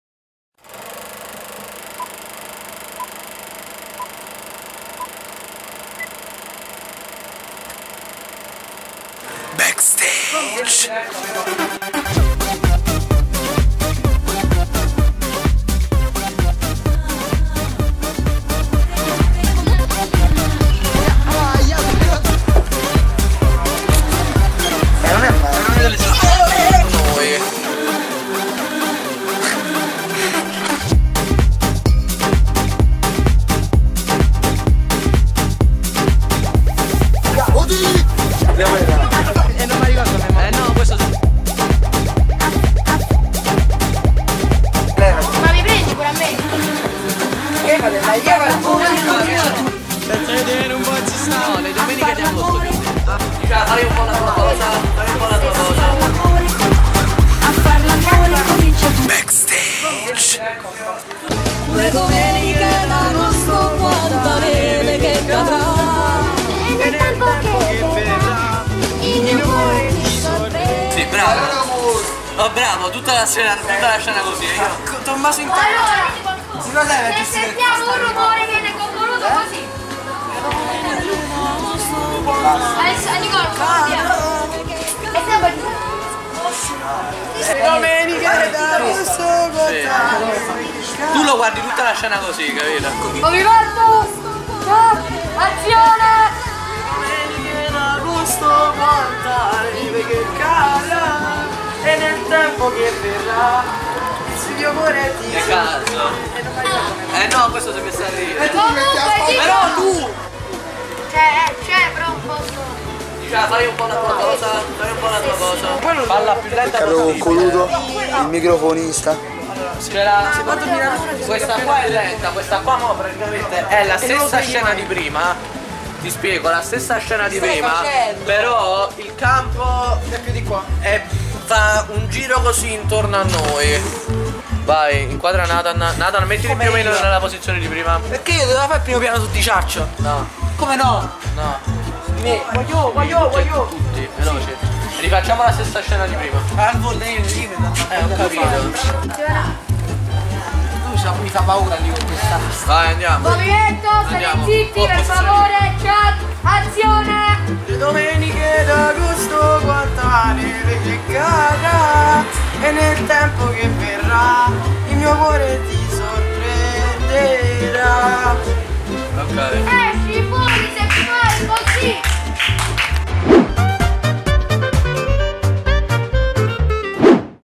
BACKSTAGE - Dietro le quinte di 'Studenti ma non troppo' [EXTRA]
Ecco a voi del materiale inedito e mai visto fin ora! Dovete sapere che proprio ieri 4 aprile 2023 a distanza di 3 anni quasi, è stato ritrovato questo materiale ancora mai usato, erano proprio riprese di backstage risalenti al 23 maggio 2020! Se avete visto il film, non c'è neanche bisogno che vi si dica a quale esilarante scena appartenga questo 'dietro le quinte'... Godetevi questi 3 minuti di video e scoprite cosa c'era dietro le scene di 'Studenti ma non troppo'!